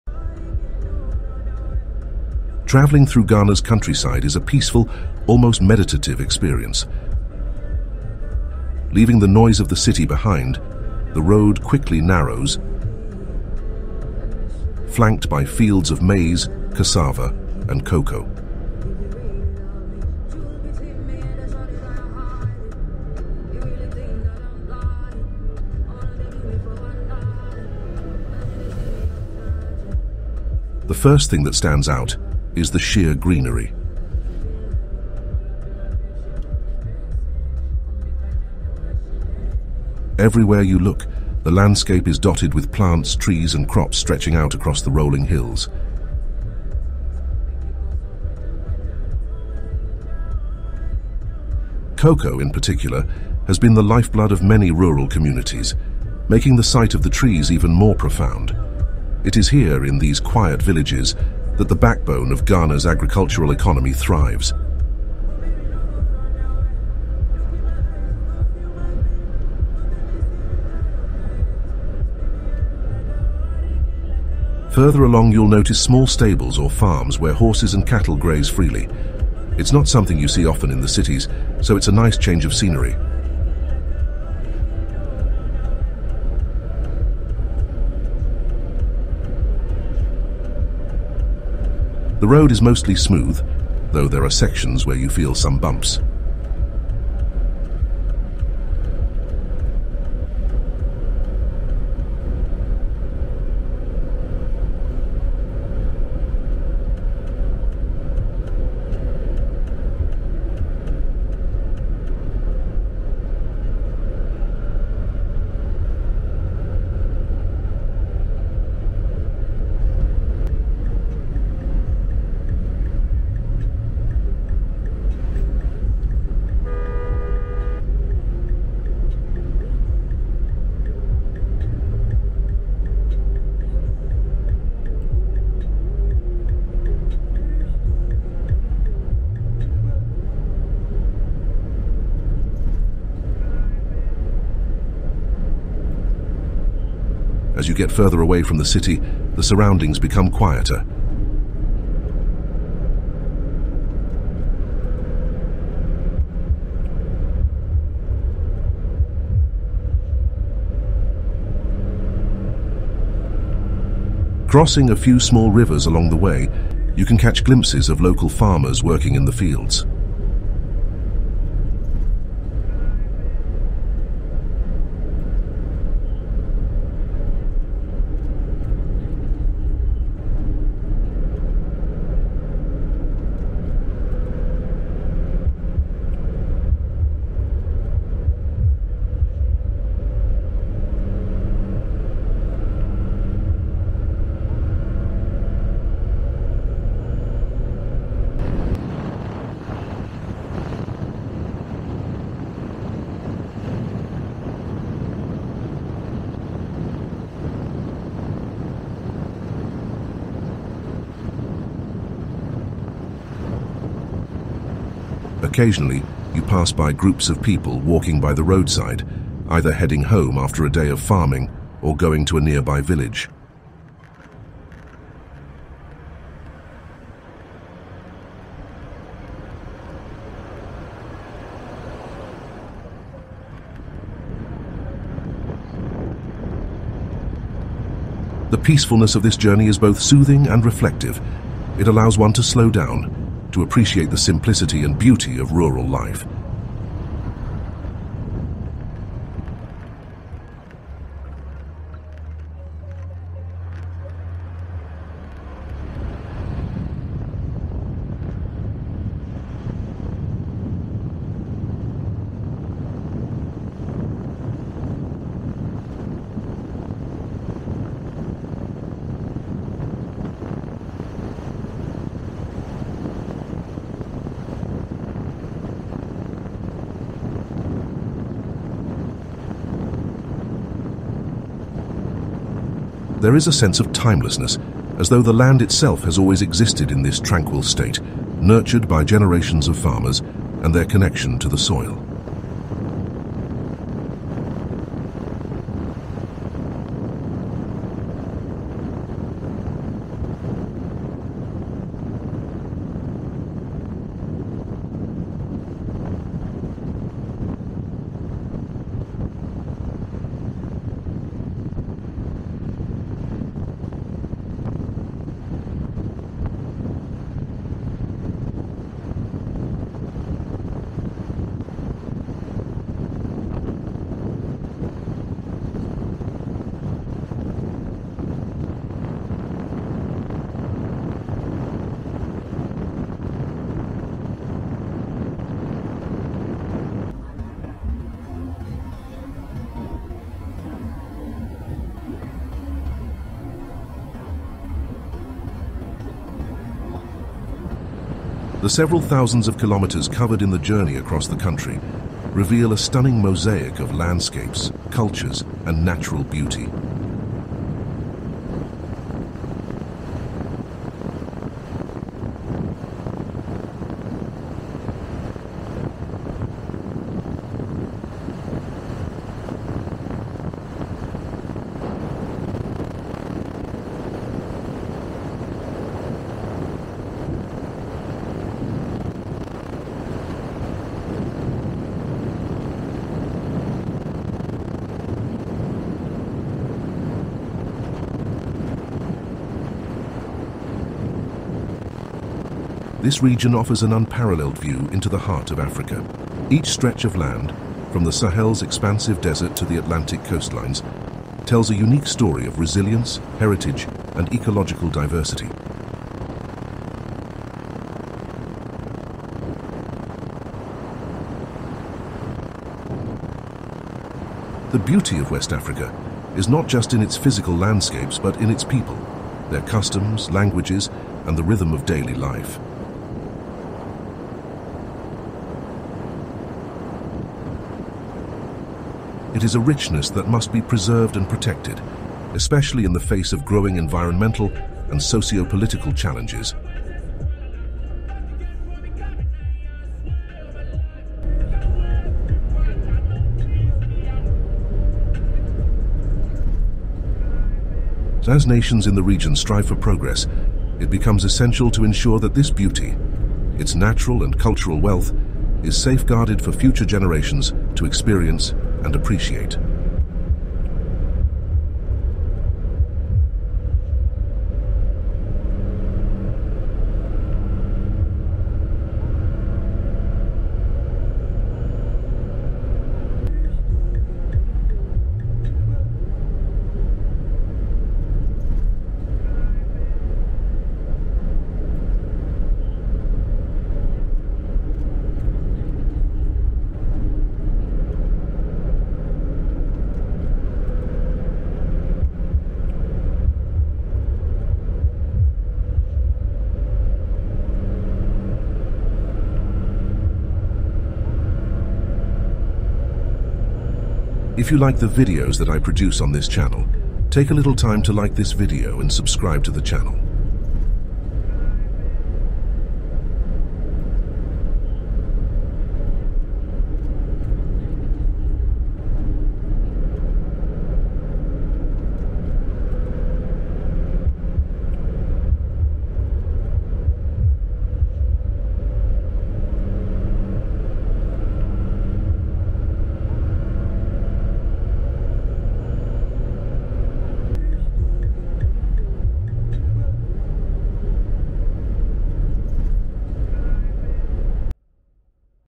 Deep Inside Ghana West Africa
Travelling through Ghana’s countryside is a peaceful, almost meditative experience. Leaving the noise of the city behind, the road quickly narrows, flanked by fields of maize, cassava, and cocoa. The first thing that stands out is the sheer greenery. Everywhere you look, the landscape is dotted with plants, trees, and crops stretching out across the rolling hills.

Cocoa, in particular, has been the lifeblood of many rural communities, making the sight of the trees even more profound. It is here, in these quiet villages, that the backbone of Ghana's agricultural economy thrives.

Further along, you’ll notice small stables or farms where horses and cattle graze freely. It’s not something you see often in the cities, so it’s a nice change of scenery.

The road is mostly smooth, though there are sections where you feel some bumps. As you get further away from the city, the surroundings become quieter.

Occasionally, you pass by groups of people walking by the roadside, either heading home after a day of farming or going to a nearby village.

The peacefulness of this journey is both soothing and reflective. It allows one to slow down, to appreciate the simplicity and beauty of rural life. There is a sense of timelessness, as though the land itself has always existed in this tranquil state, nurtured by generations of farmers and their connection to the soil.